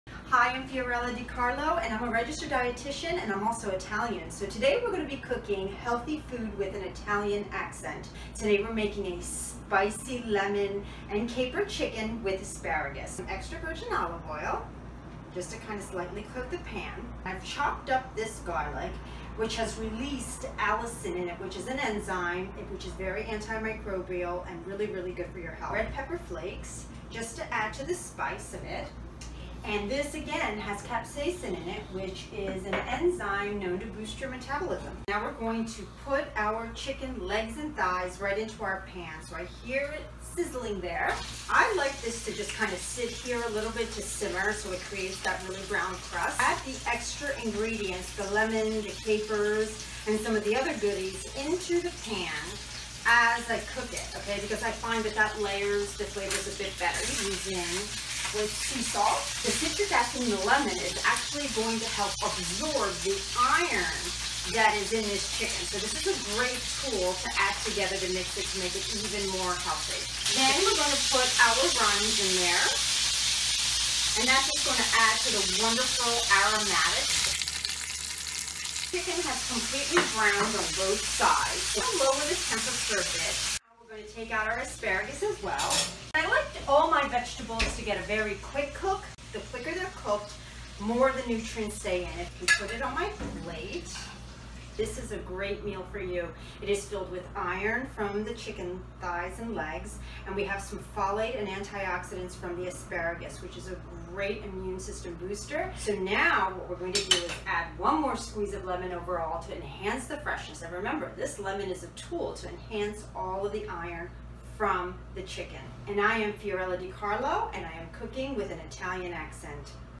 Spicy Lemon Caper Chicken with Asparagus--FiorellaEats
Fiorella DiCarlo is a Registered Dietitian who loves to cook with an Italian accent! This recipe uses a combination of fresh, seasonal ingredients that work together to make a dish full of flavor and packed with nutrition!